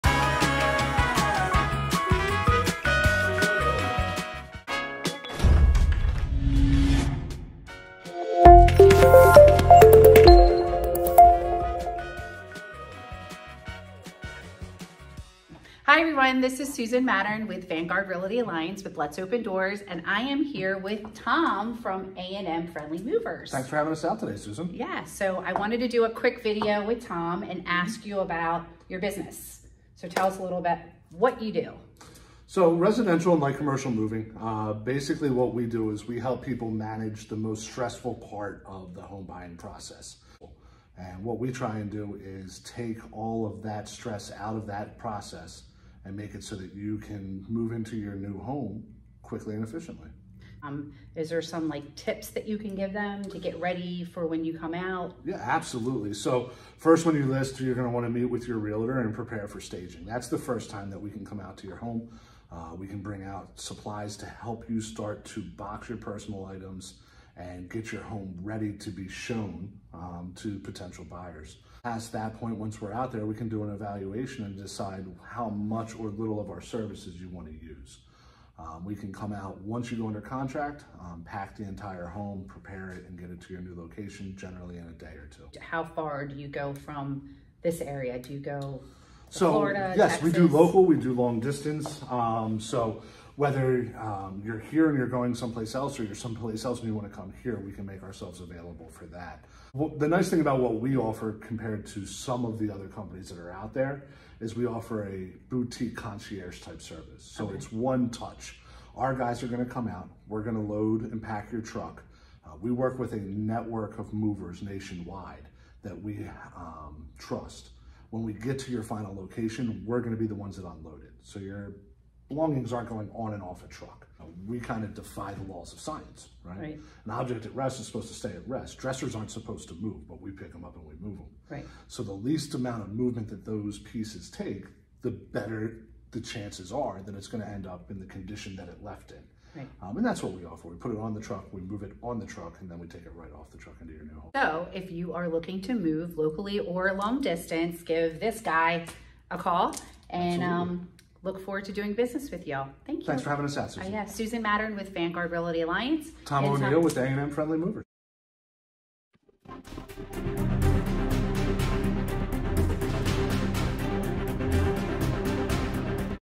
Home Seller’s Workshop- looking for friendly movers? Got A & M Friendly Movers…
See what A &M Friendly Movers offers their clients … it’s a one stop shop… packing, moving, transporting and delivering your house in one piece and with a smile…😁